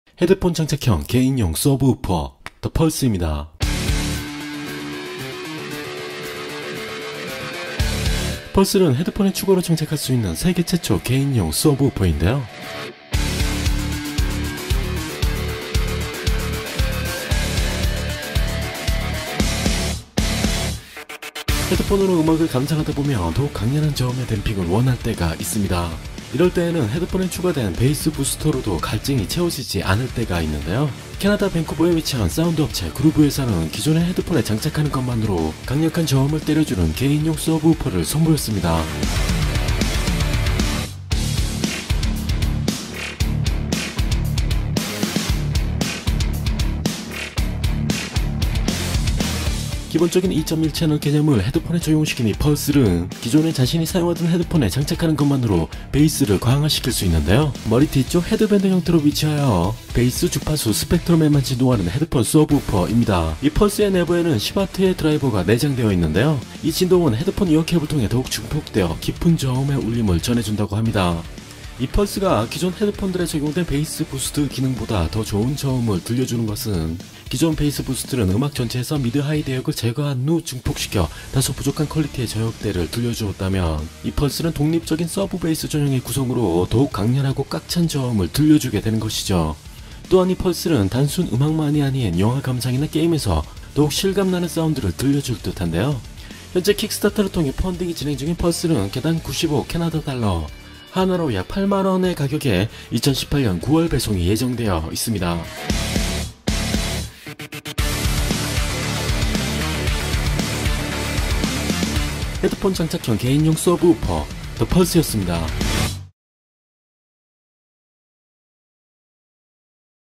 헤드폰 장착형 서브우퍼 'Pulse'-[스나이퍼 뉴스룸]
캐나다 밴쿠버에 위치한 사운드 업체 Grüve에서는 기존의 헤드폰에 장착하는 것만으로 강력한 저음을 전해주는 개인용 서브우퍼를 선보였습니다.